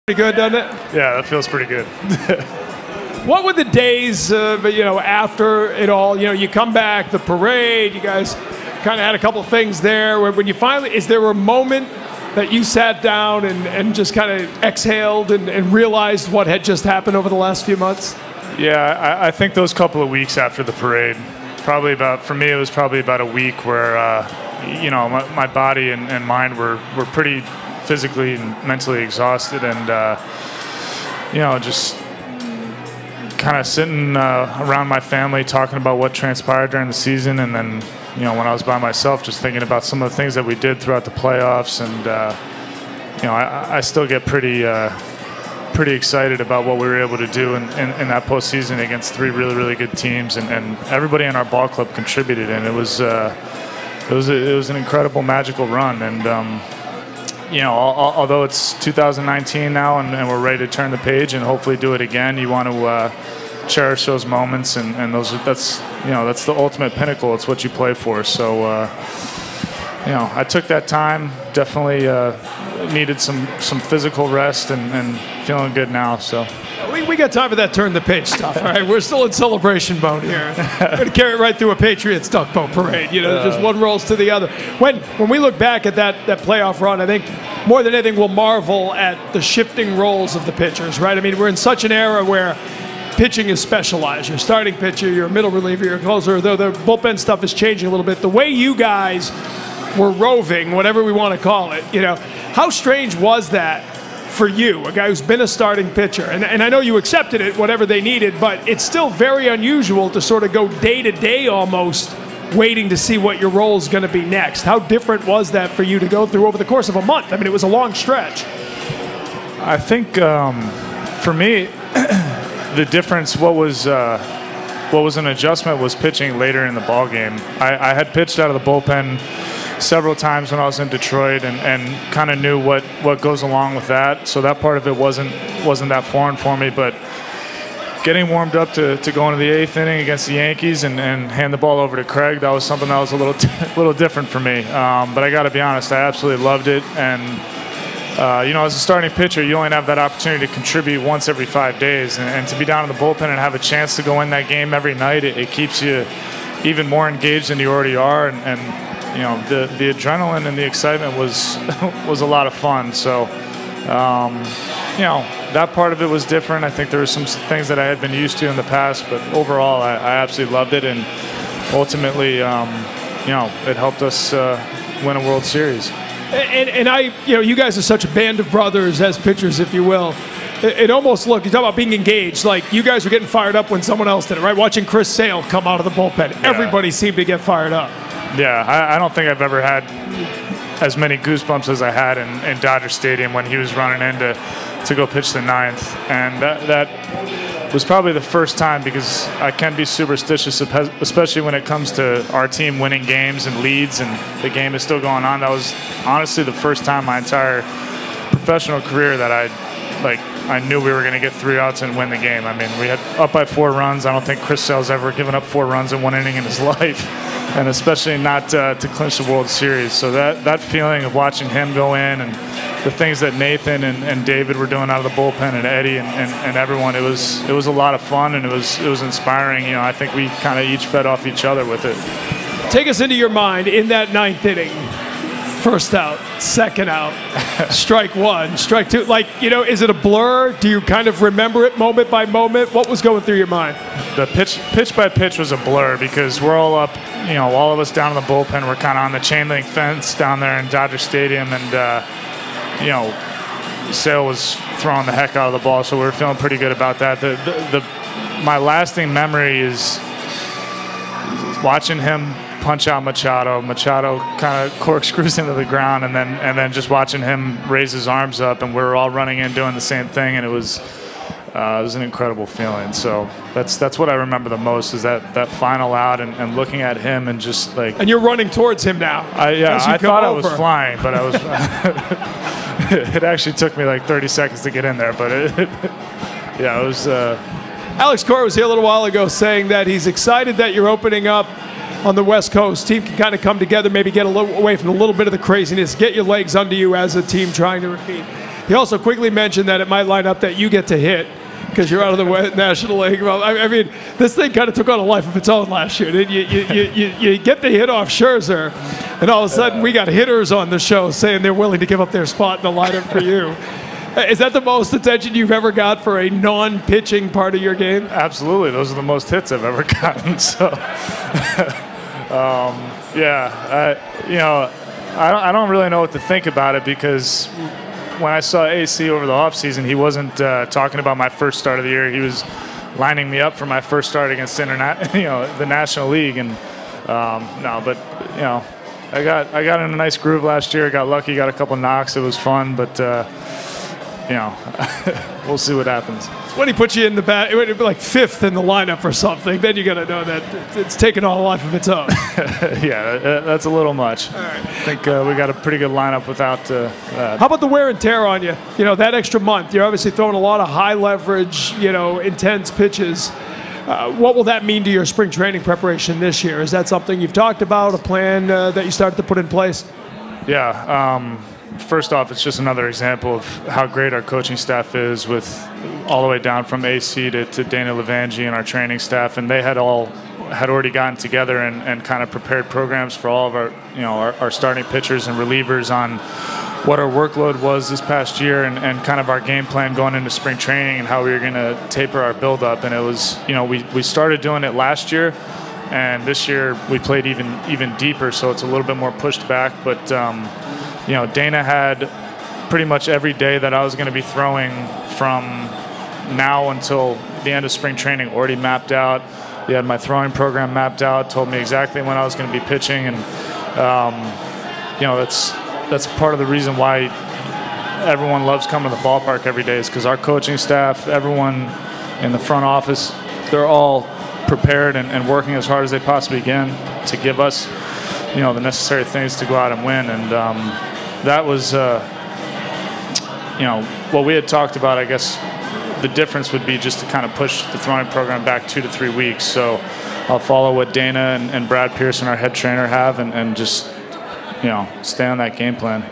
Red Sox Winter Weekend: Rick Porcello Interview
Boston Red Sox pitcher Rick Porcello spoke with NESN's Tom Caron at Red Sox Winter Weekend.